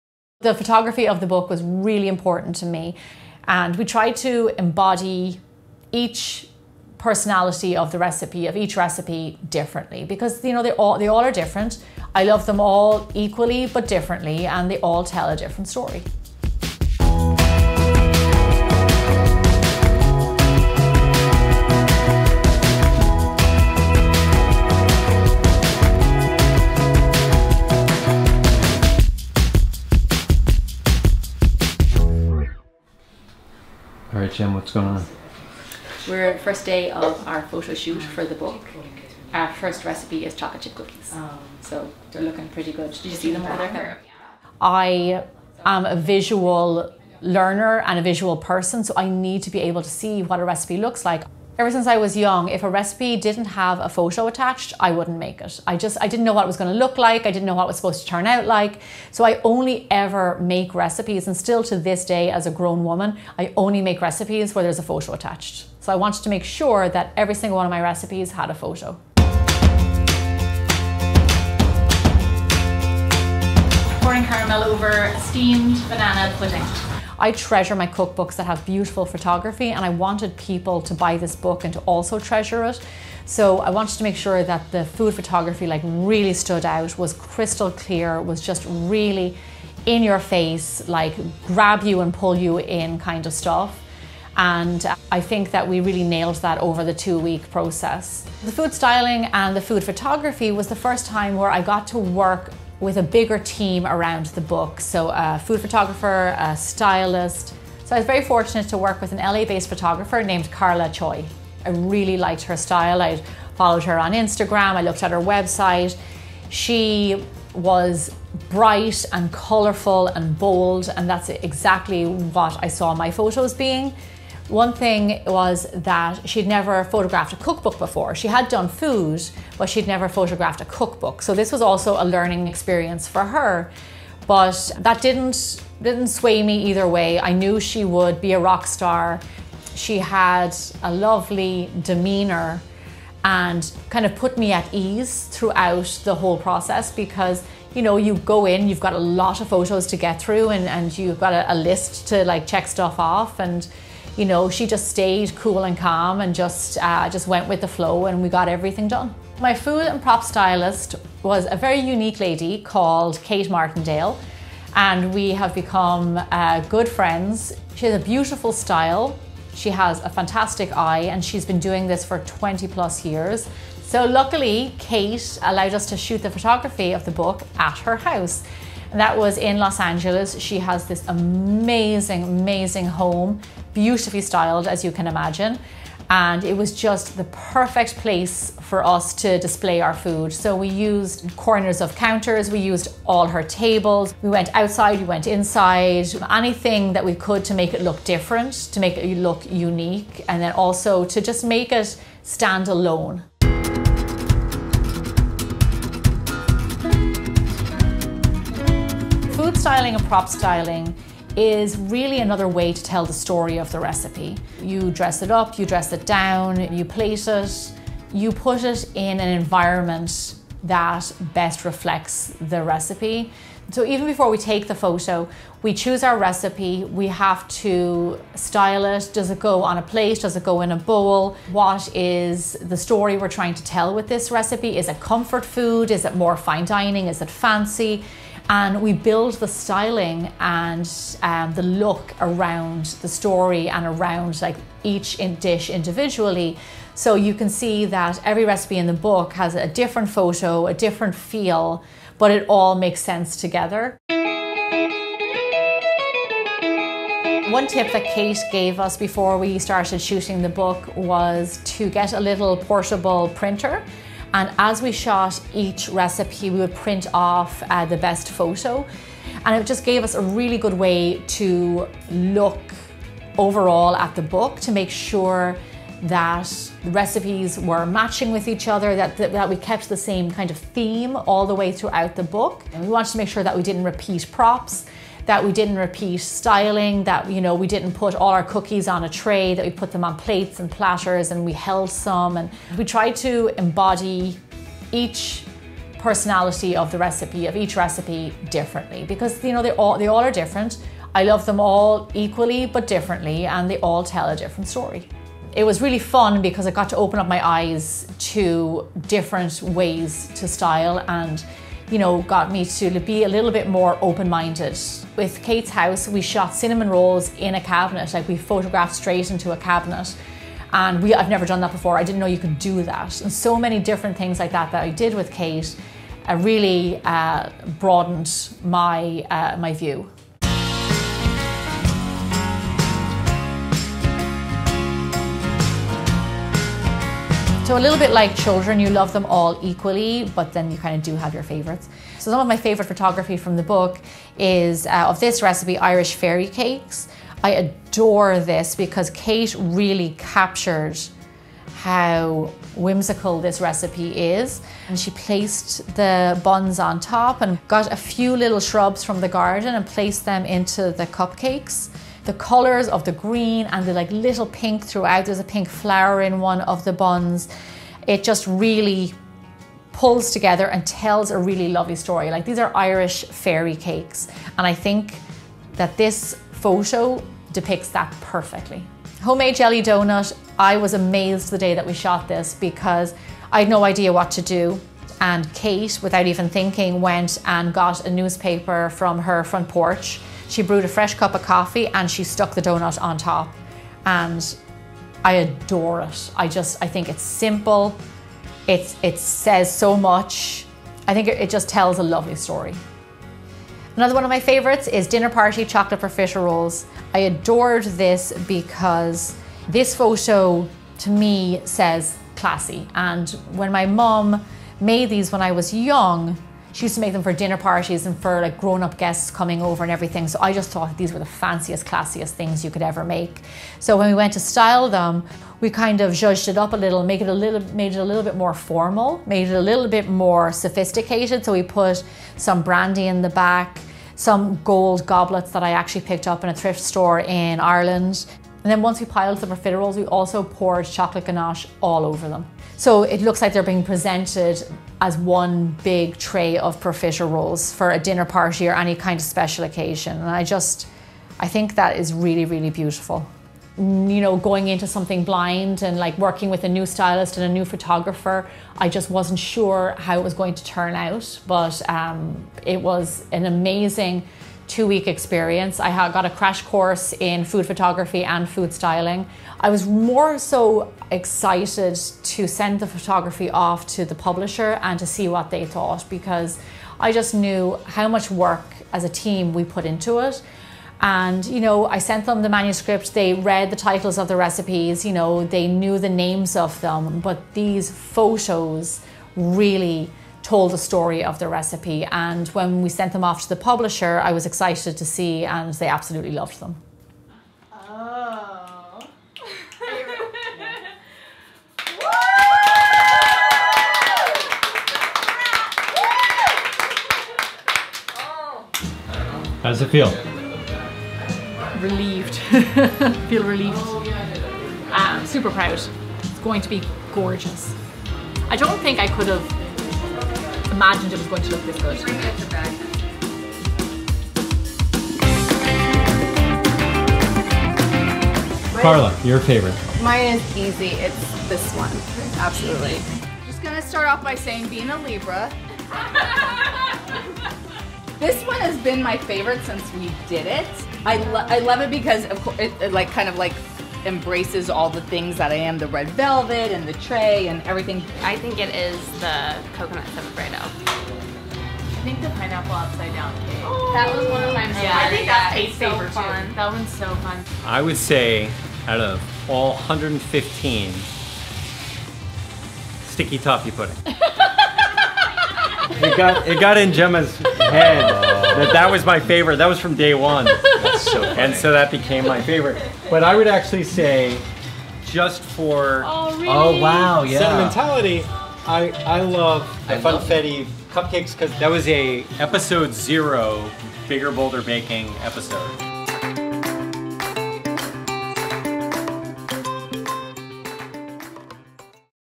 PHOTOGRAPHING the Bigger Bolder Baking Cookbook
I believe every recipe needs a photograph so there’s one for each of the 115 recipes in my cookbook. Watch how the photography came together. Pre-order my cookbook now! Amazon

Gemma Stafford | Bigger Bolder Baking